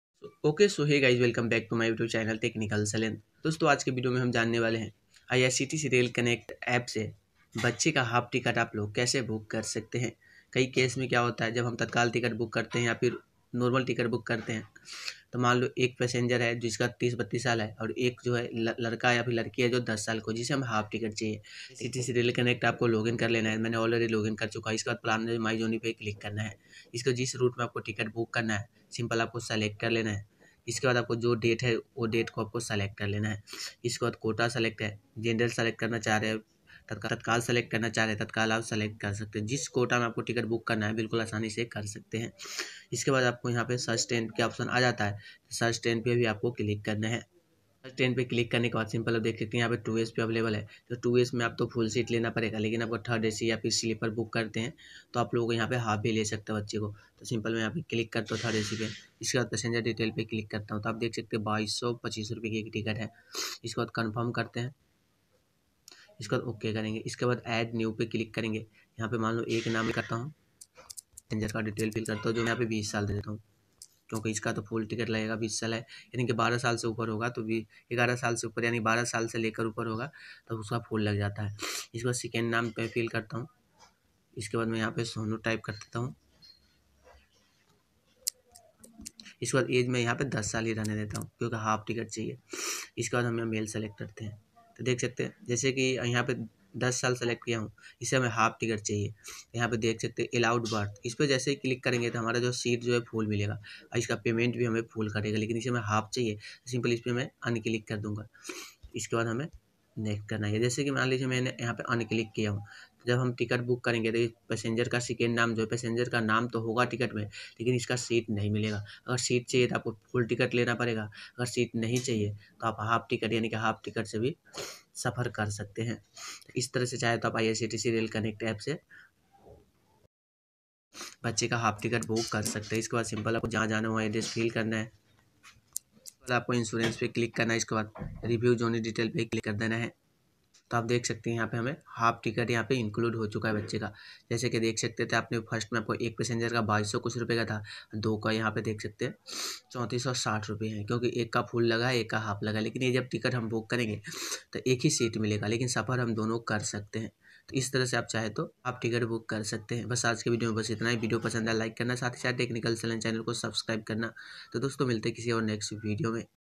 Who To book Half Train Ticket Form IRCTC Online। IRCTC Half Train Ticket Booking 2021
How To Book Half Train Ticket From Irctc Online

►your question

My Camera :-